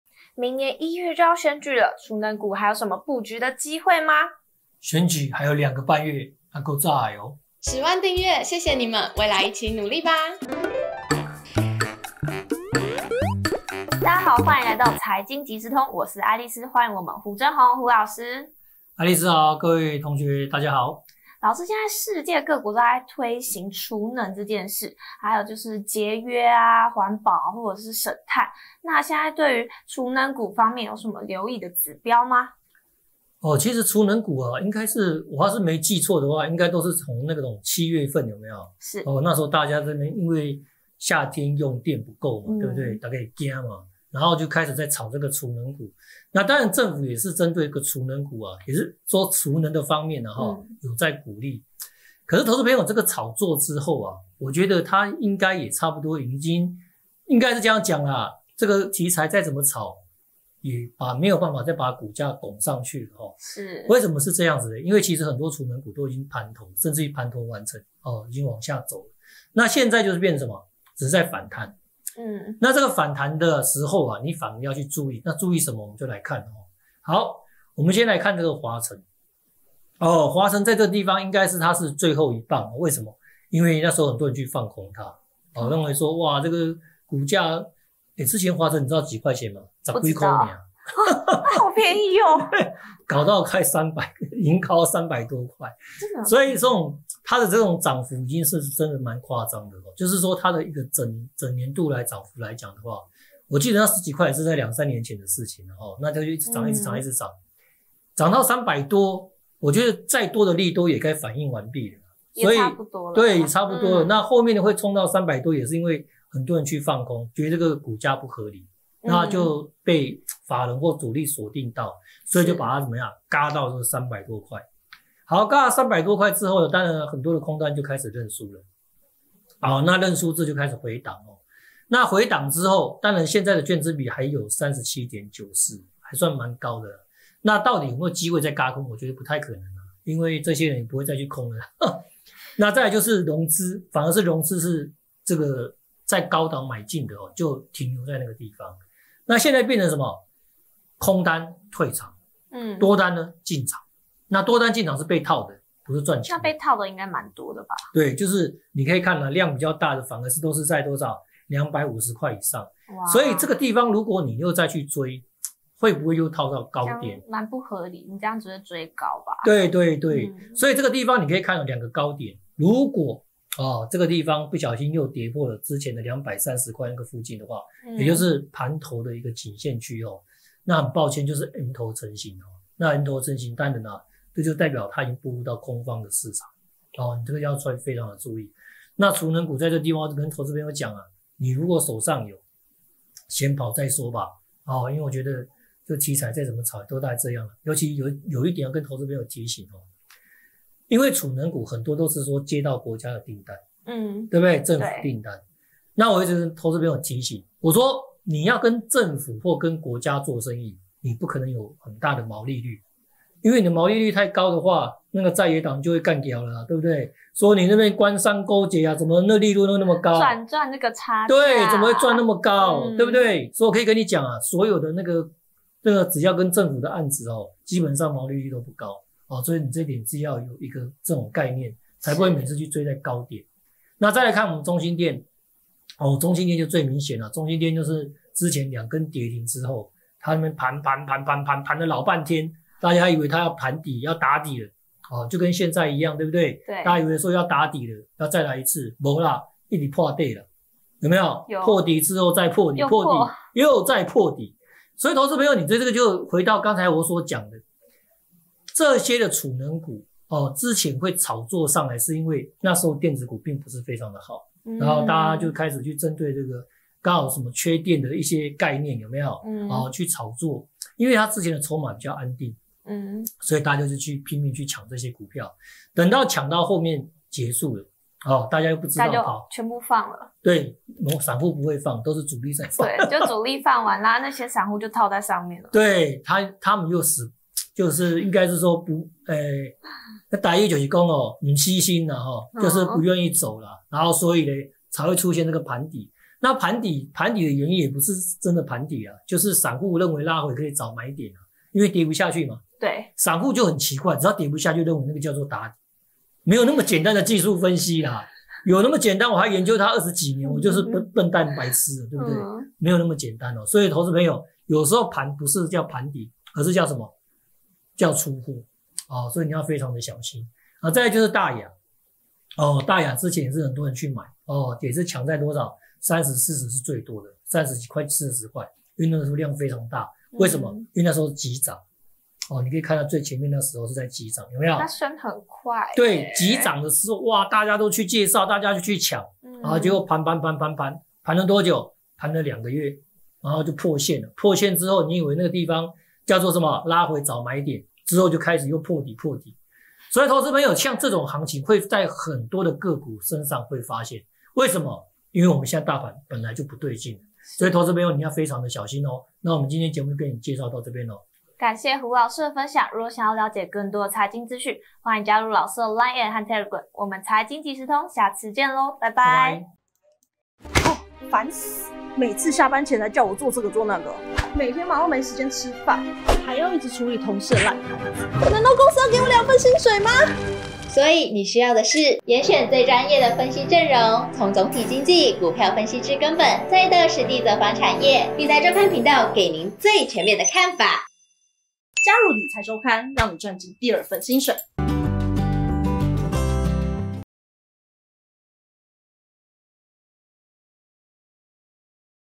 財金即時通-20231030／選舉行情近了？！ 儲能股發光誰最耀眼？
隨著時間的推移⏳
各個候選人都在為選票加緊步伐👣
選舉到底會帶來怎樣的趨勢呢⁉
快來聽聽胡老師怎麼說👀
🔥開課啦！更多財經消息都在【股市同學會】
🔥美企財報超預期 AI股這次彈真的？